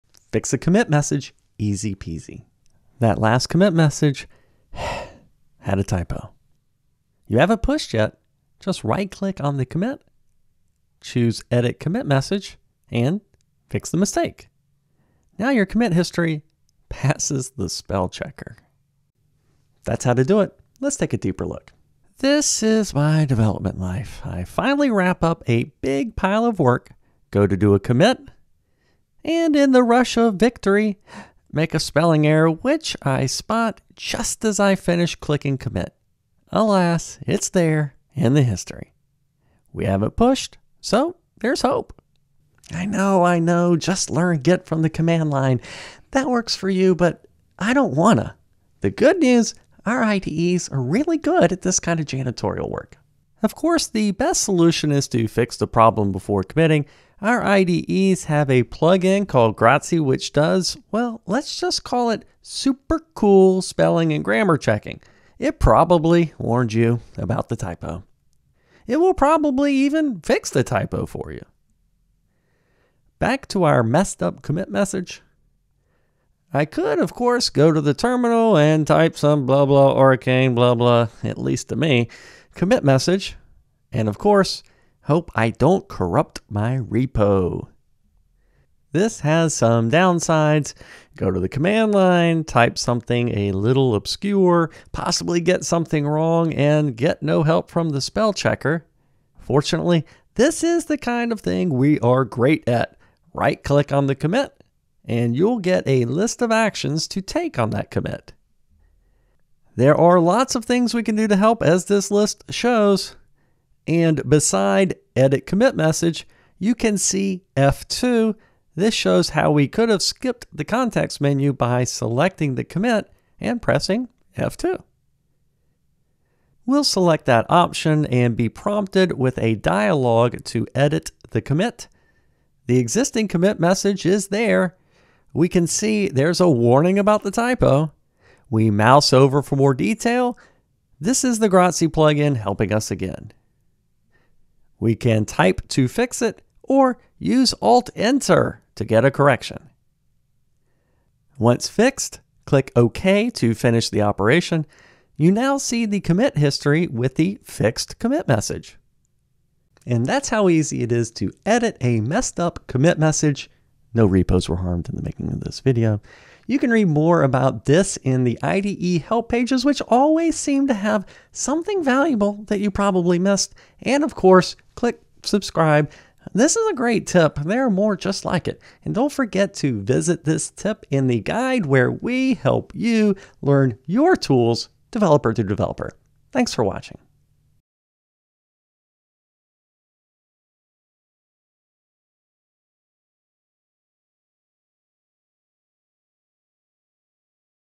Edit Commit Message in any JetBrains IDE
Found a typo in a commit message that you just committed?

You can still reword it before you push it. Watch how Paul Everitt handles this in just 15 seconds! The video then explores how to do the same in the terminal, what shortcut you can use to edit your commit message, and how your JetBrains IDE helps prevent spelling errors in the first place.